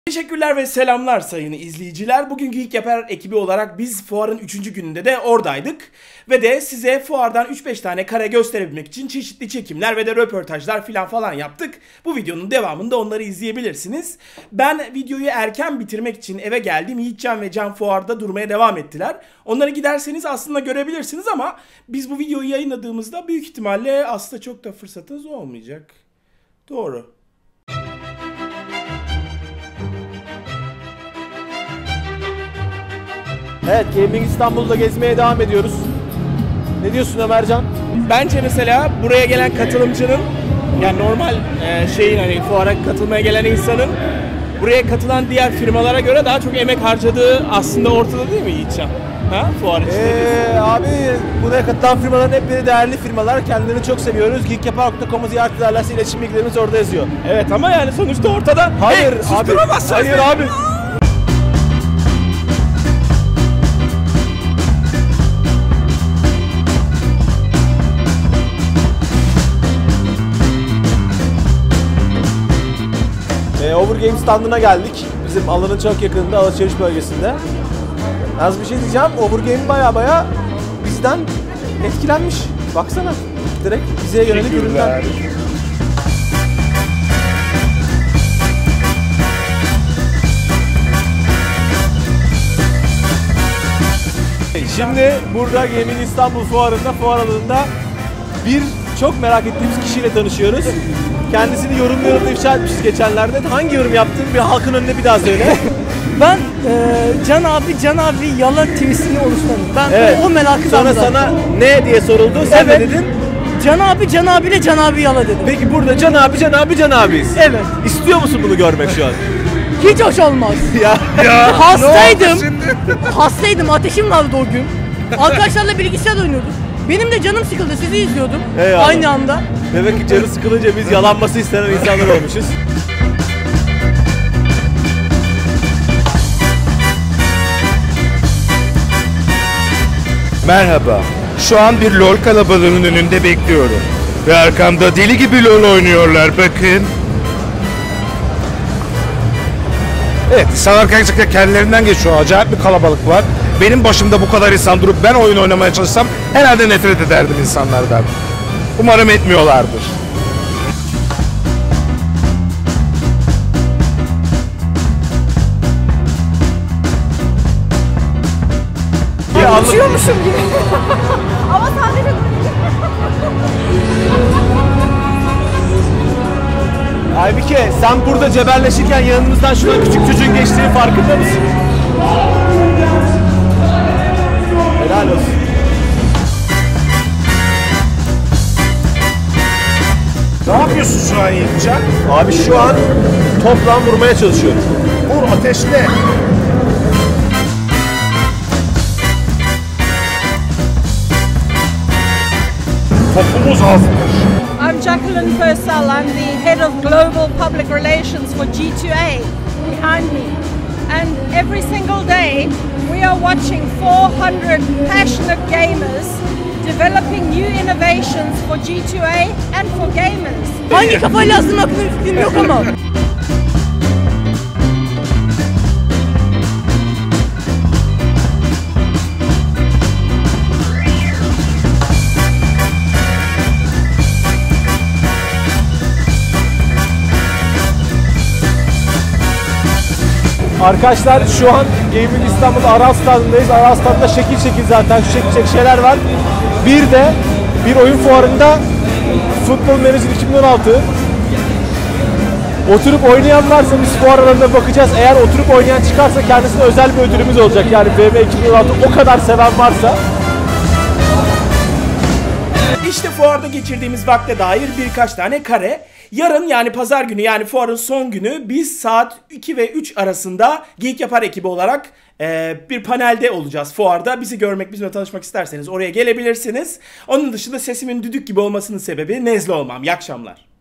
GIST'ten ORTAM Manzaraları!
Geeklive GIST'i sizlere göstermeye uçaradım devam ediyor! Geekyapar ekibi fuarda çılgın attı, bir sürü insanla tanıştı, danalar gibi eğlendi. İzleyin bakın bizim Can'lar ve Aybike nasıl da eğlenmiş!